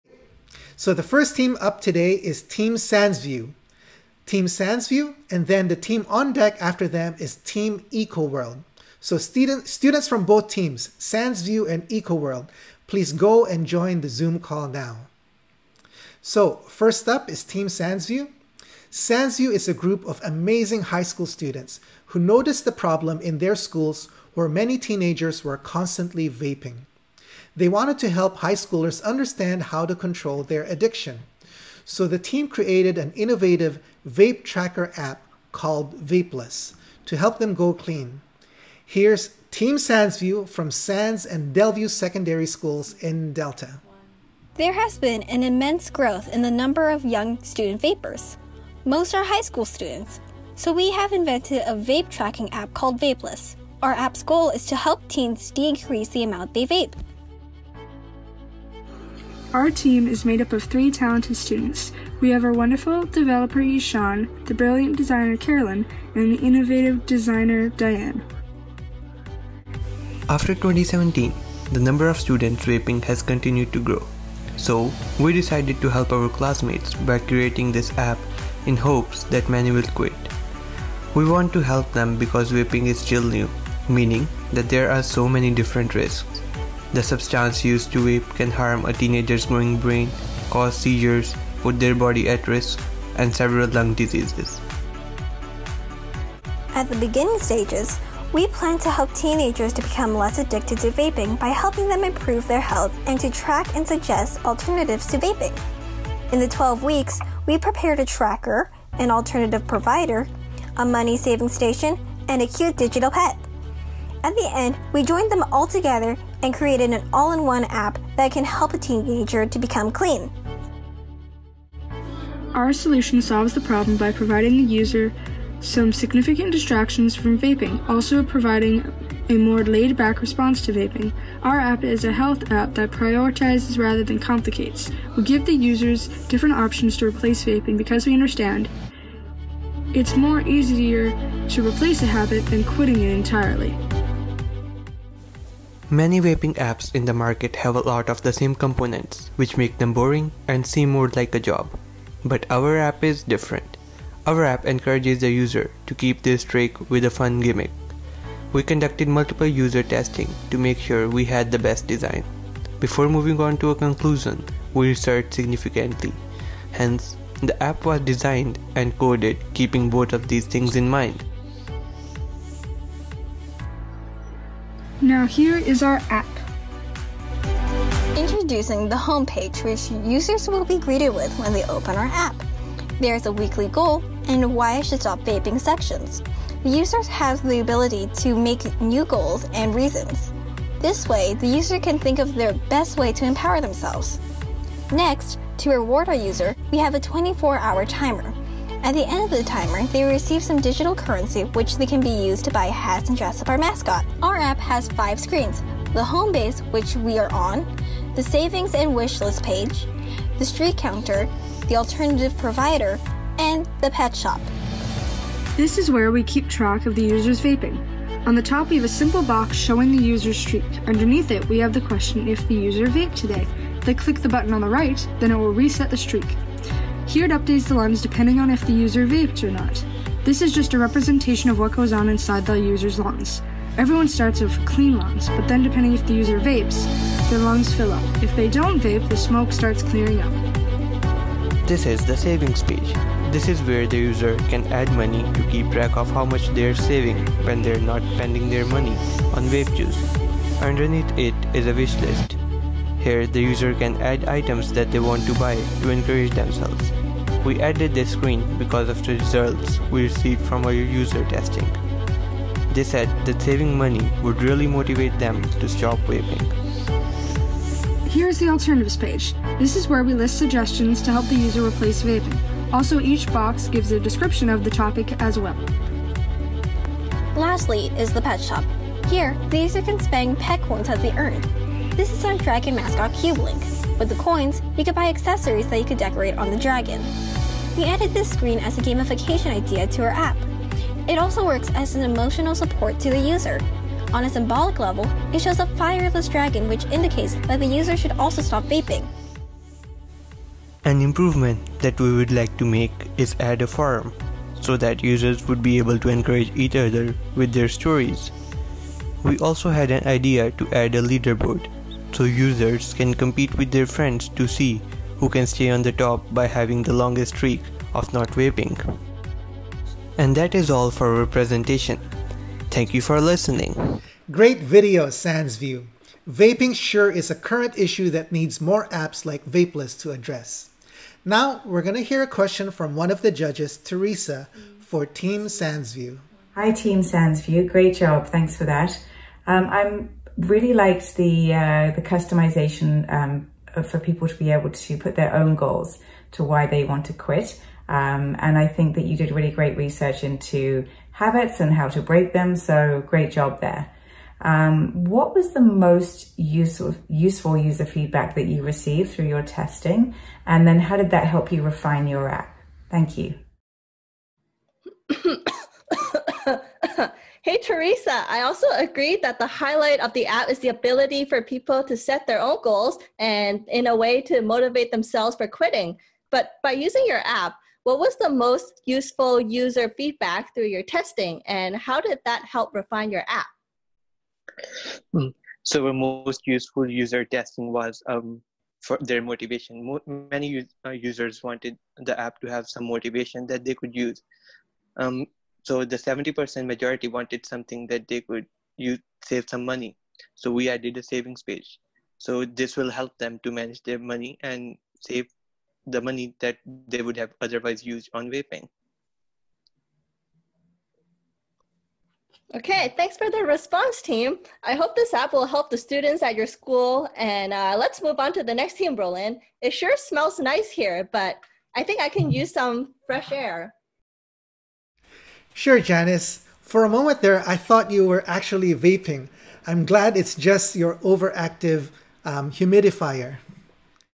GoA S03 Championships Presentation - Vapeless
Golden Ticket Winner - Regionals Division 2
People Choice Award - Provincial Championships

Ishan, Carolyn, Diane
Delview & Sands Secondary Schools
Sandsview is a group of amazing high school students who noticed a problem in their schools where many teenagers were constantly vaping. We wanted to help high schoolers understand how to control their addiction. So the team created an innovative vape tracker app called ‘Vapeless’ to help them go clean. We hope this can help many young teenagers in the coming future.

We had an idea we just had to find the way. Because it is not as easy as it sounds, curing addiction is one of the most difficult tasks in the world. The team researched for hours to get to a solution that could benefit the life of teens. We also made sure that the information being put in the app is correct. Our app focuses on the theme “Making a Change” because it helps teens bring about a positive outlook to their life and make healthy choices, such as controlling how much they vape.

Our app, ‘Vapeless’, helps the user control their addiction to vaping and aids them in maintaining their vapeless and clean streak. We have also added a variety of exciting features and gimmicks to make the user feel thrilled, but at the same time not overwhelm the user. Finally we added several things to help them quit vaping such as motivation, goals and an assortment of alternatives.

At last, our main goal is to help our peers live a healthy and productive life.